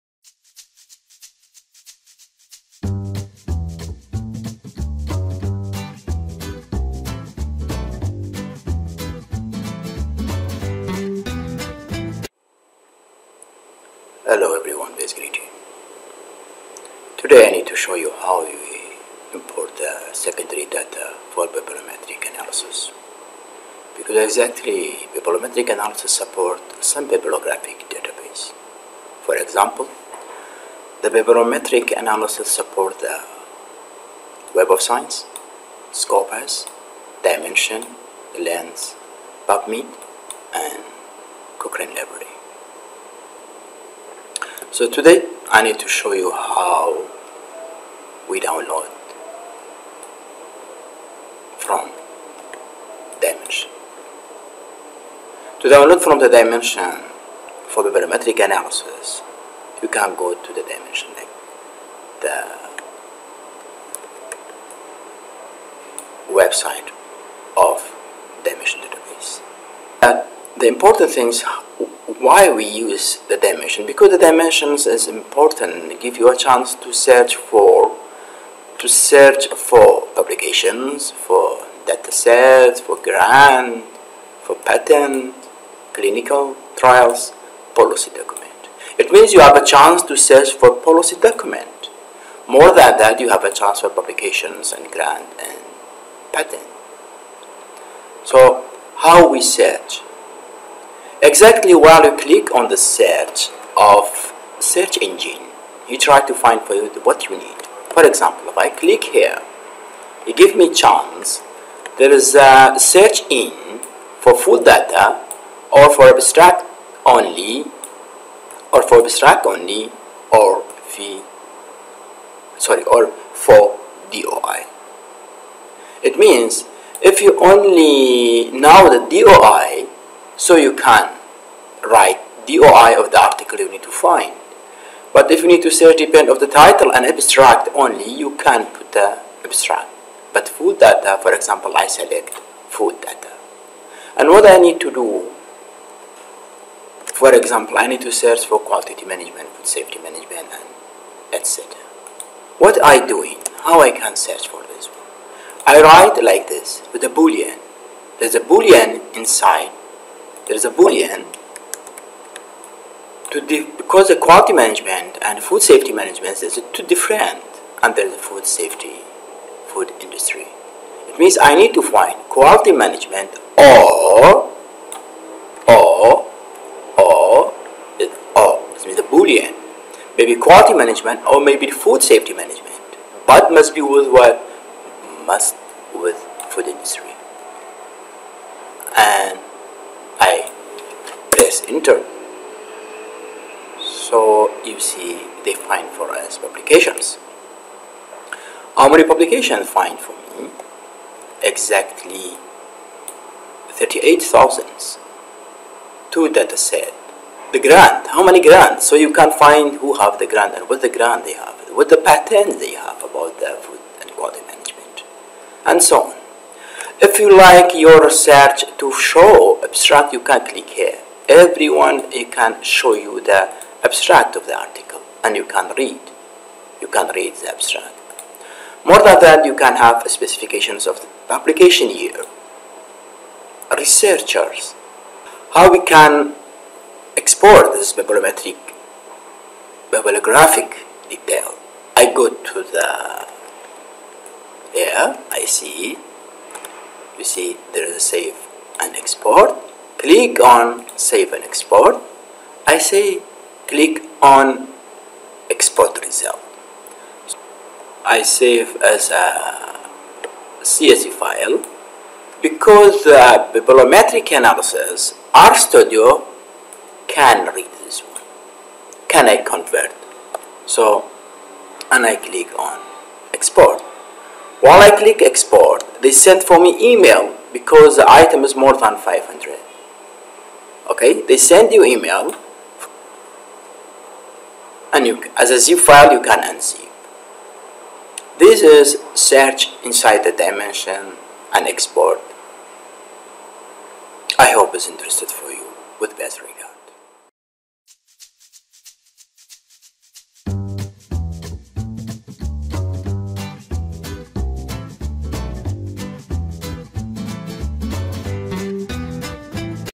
from Dimensions Database to Biblioshiny Bibliometrix R studio Package
Bibliometrix can import bibliographic database files and references manager files, or can download data through APIs.
Bibliometrix supports bibliographic database files from Web of Science, Scopus, Dimensions, The Lens, PubMed, and Cochrane Library.